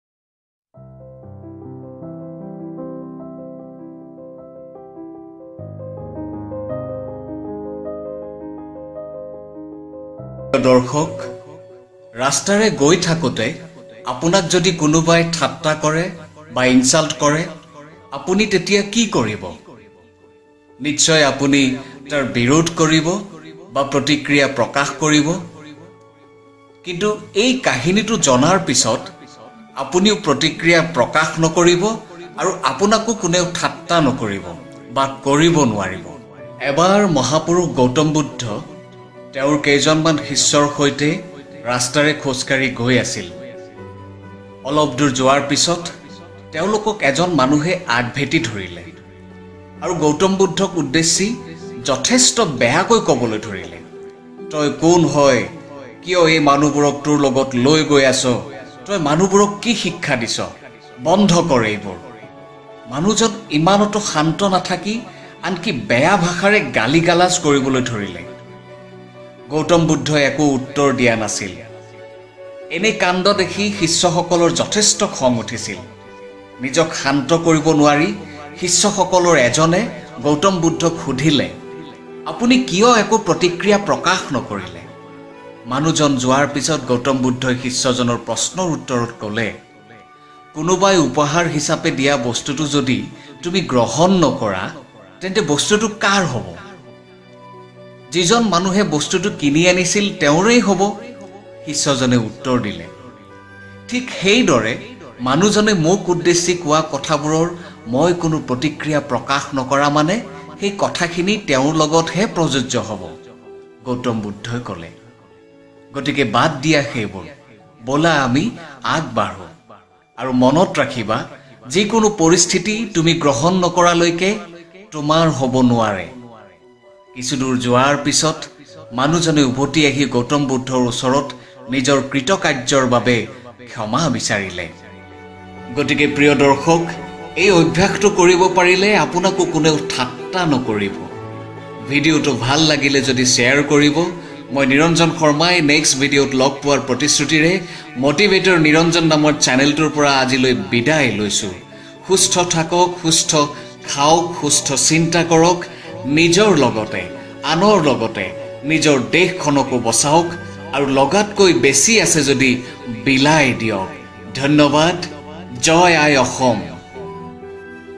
No One Can Insult You - How to react when some one insults You - by Niranjan | Motivator Niranjan
About this video:
A moral story on behavior.